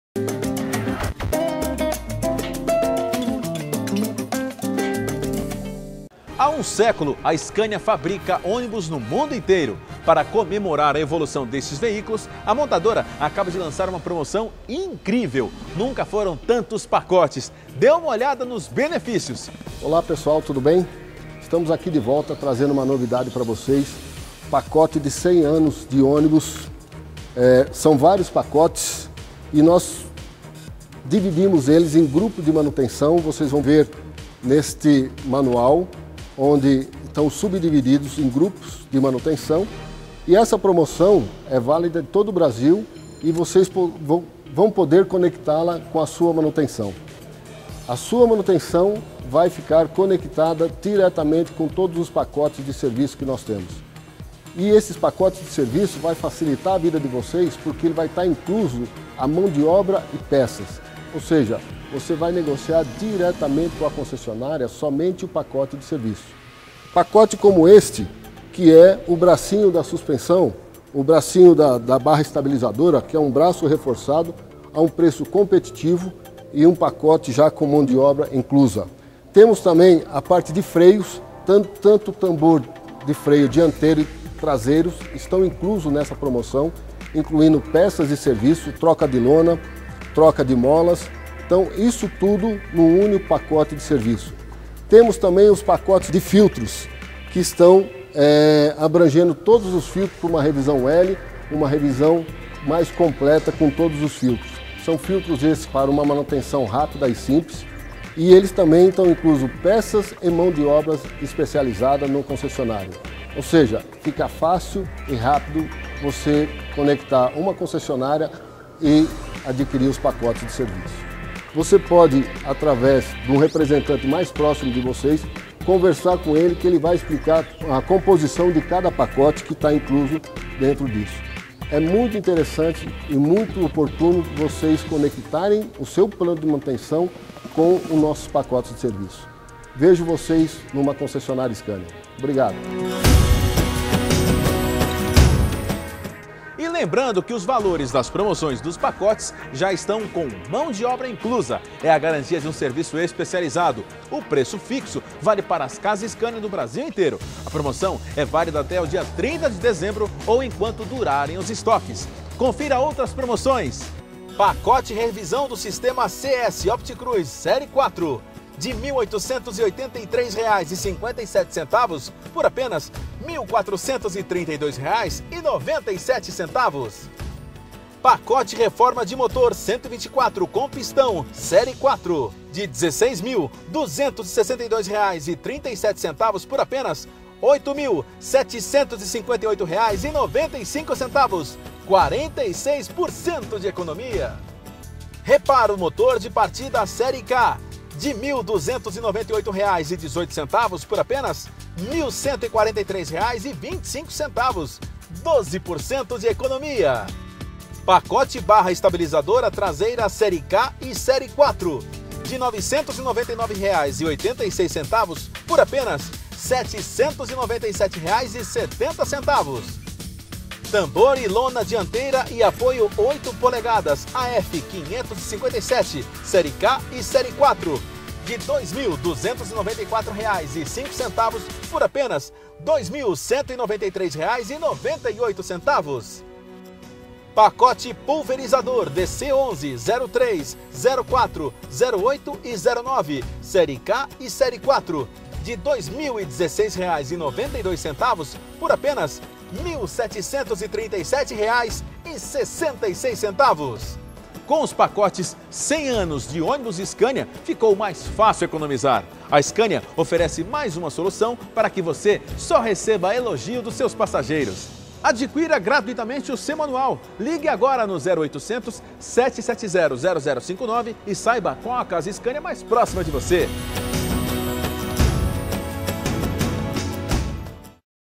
Rede Scania na TV - 11/12/11 - Pacotes 100 anos Ônibus
Aproveite os preços especiais dos Pacotes com Preços Fixos em comemoração aos 100 anos dos ônibus da Scania no mundo. São mais de 60 kits com peças Scania e mão de obra especializada inclusa com preço fixo em todas as Casas Scania do Brasil.